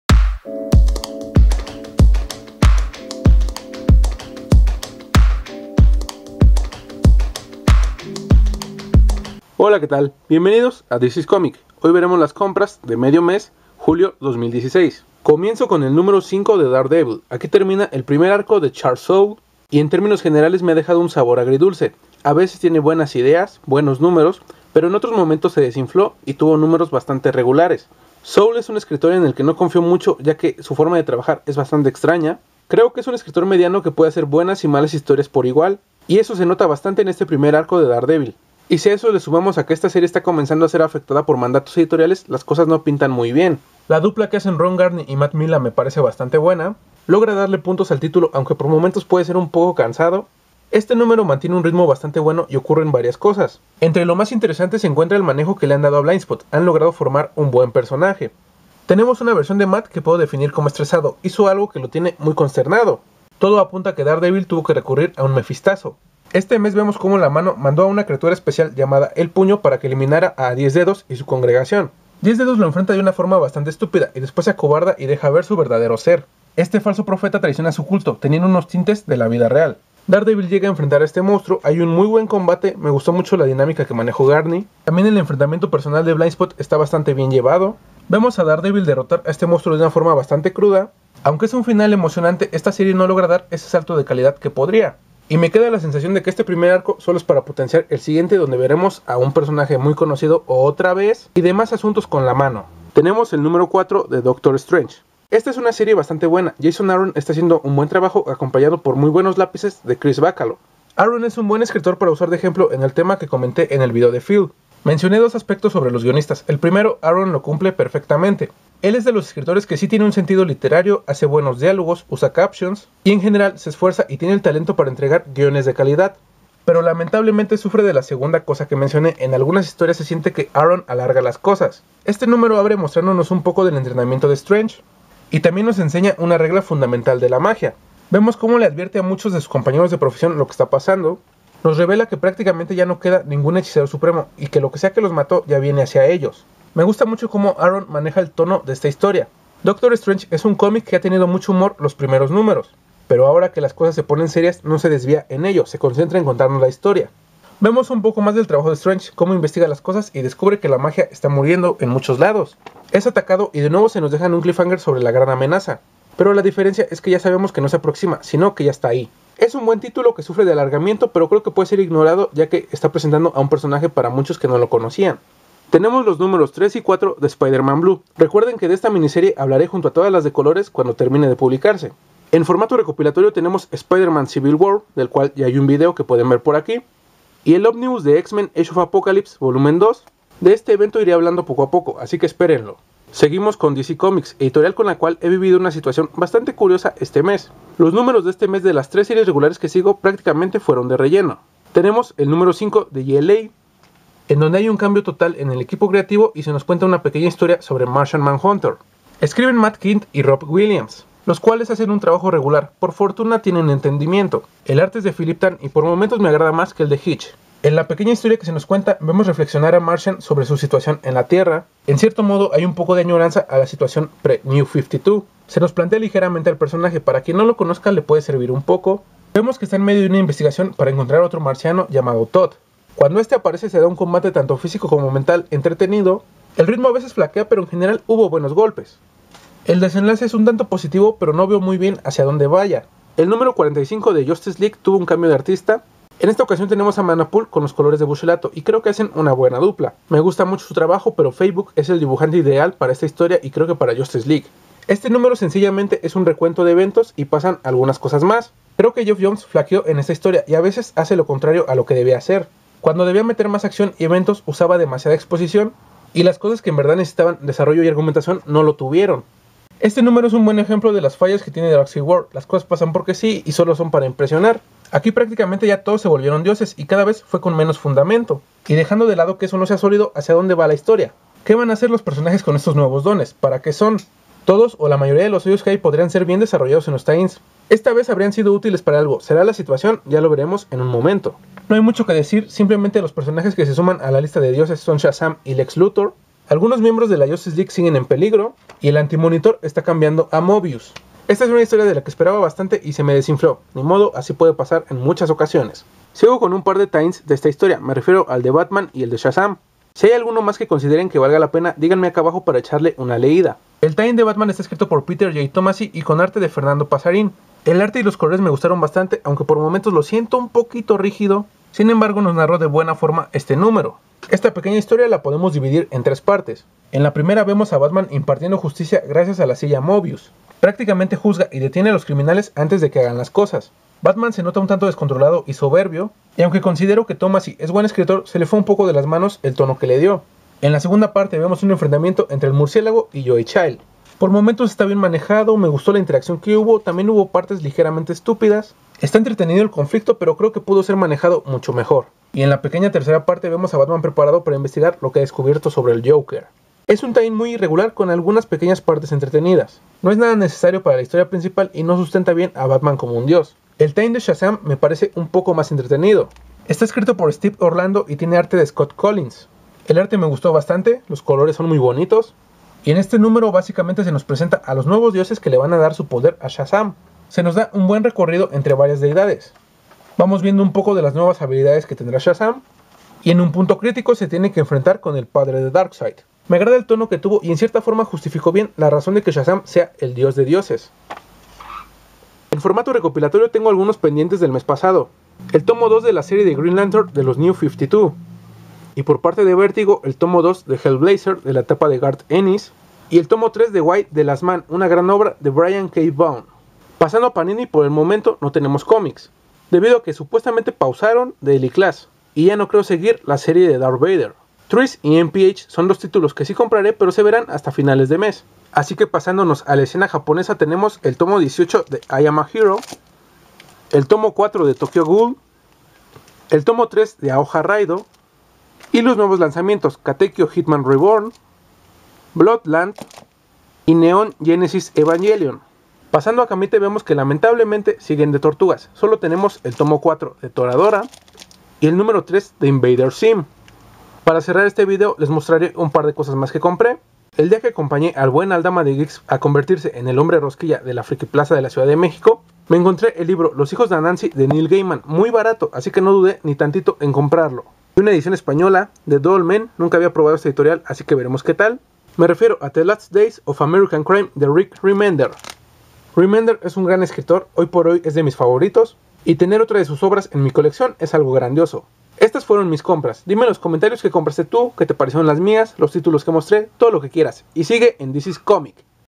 Compras de medio mes Julio 2016
Fernando muestra las primeras compras del mes, desde Soule hasta Remender...